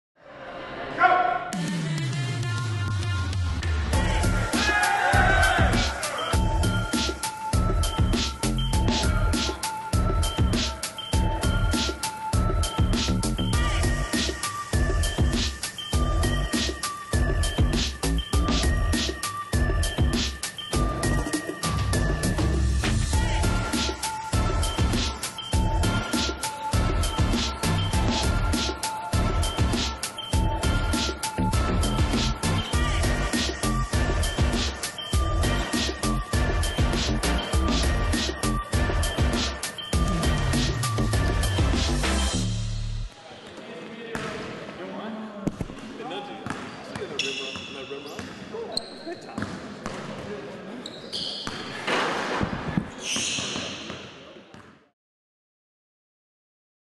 Iowa Men's Basketball Summer Workouts
The Iowa Men's Basketball team is hitting the weight room preparing for the upcoming season.